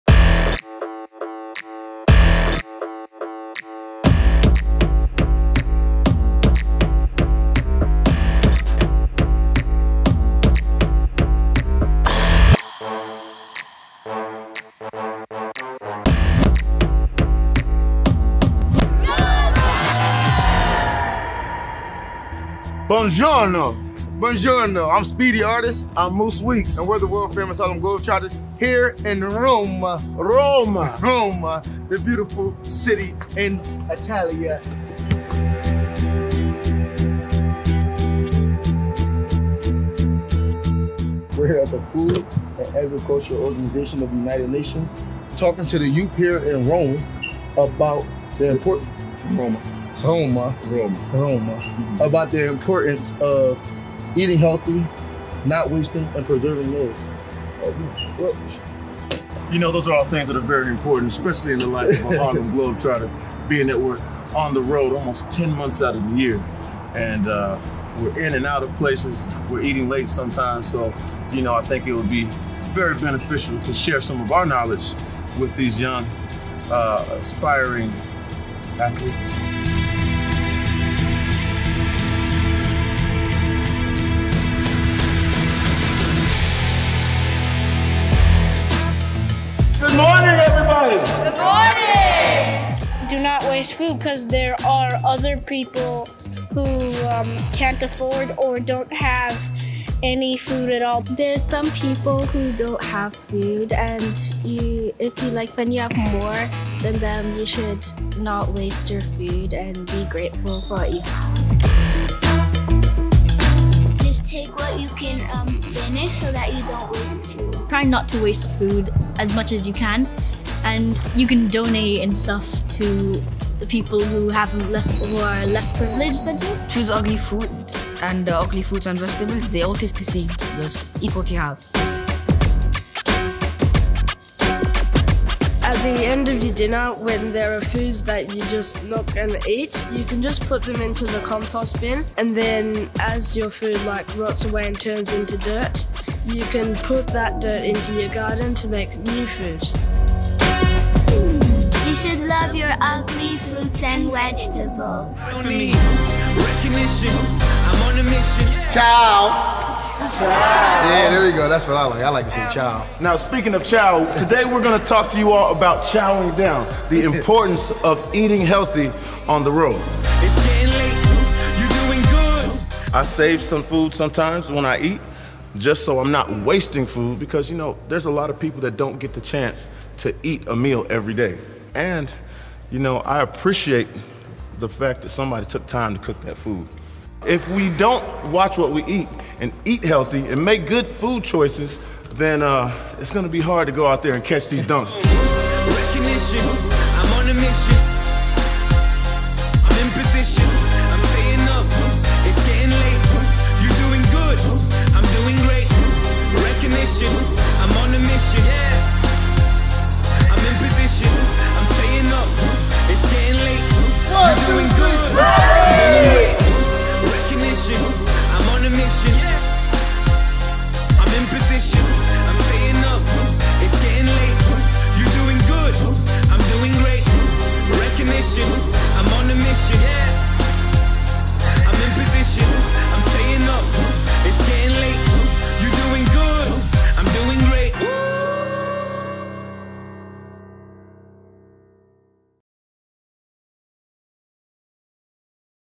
哈林篮球队携手孩子们共同对抗食物浪费
3月10 日美国篮球表演队哈林花式篮球队球员与意大利首都罗马当地学生共同到访联合国粮食及农业组织（粮农组织）总部，参与“推广健康膳食、减少粮食浪费”宣传活动。绰号“Speedy”的Darnel Artis 和绰号“驼鹿”的Alex Weekes借此机会与罗马的学生们讨论了保持健康饮食的重要性。学生们和球员们还谈到了减少学校、家庭、体育馆和自助餐厅粮食浪费的9个简单技巧。哈林花式篮球队球员和学生们随后离开粮农组织总部，前往体育宫，并在晚间为一万名观众表演卓越的篮球技艺。